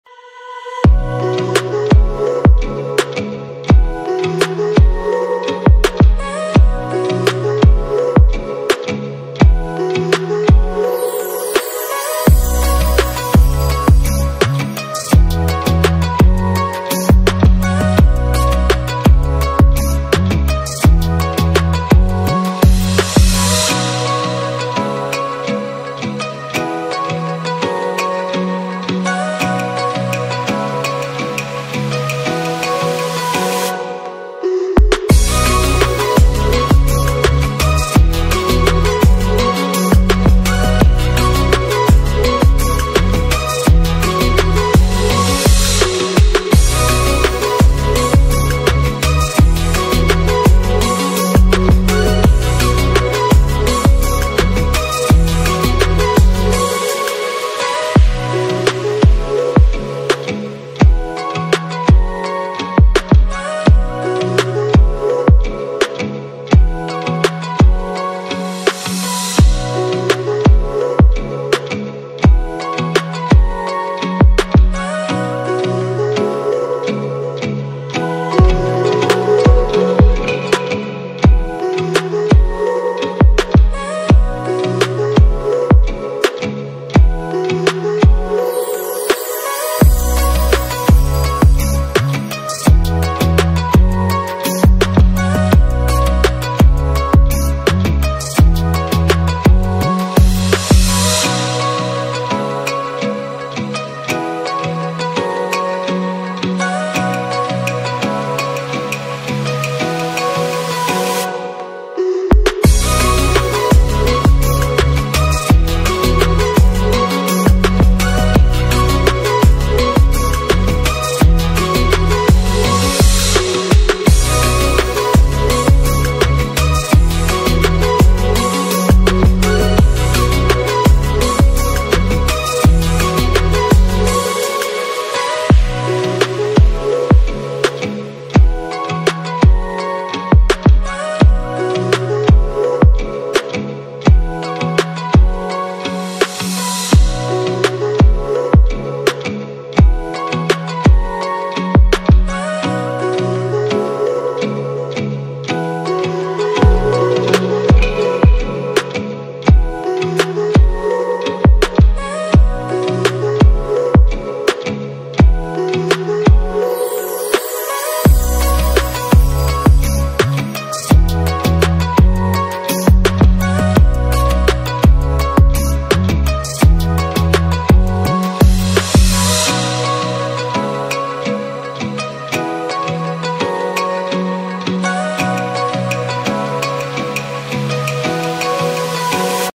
Small console table decorating!
How to decorate a small console table. quick and easy refresh, console table styling!